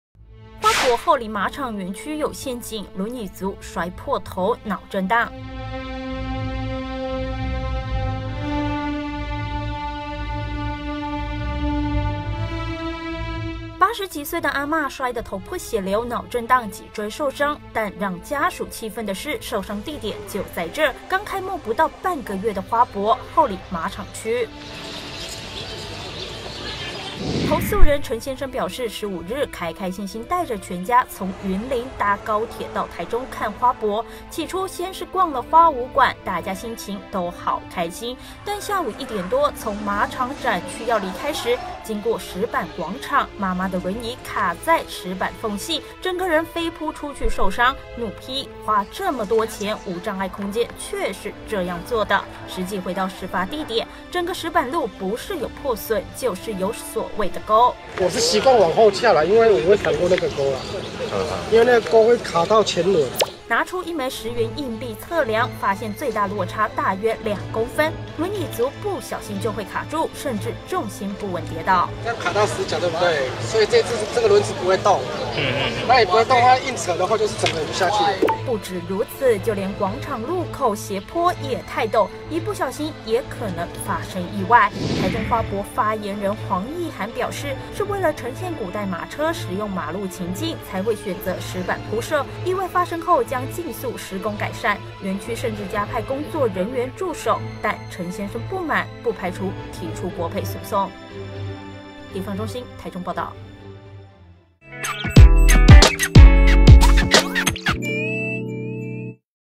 隱形殺手！輪椅婦遊花博慘摔腦震盪 家屬擬提國賠 | 台灣蘋果日報
台中花博開幕不到半個月，后里馬場園區前天下午就發生遊客受傷意外，一名80多歲老婦人坐著輪椅逛園區時，因輪椅前輪突然陷入石板路面間的空隙卡住，老婦人當場飛出、正面朝下慘摔，經送醫檢查，因有腦震盪、頸椎受傷等狀況住院觀察。老婦人陳姓兒子不滿地向《蘋果》爆料說：「花這麼多錢辦的大型活動，無障礙設計不應該這樣，應該要更周全，我不排除提國賠訴訟。」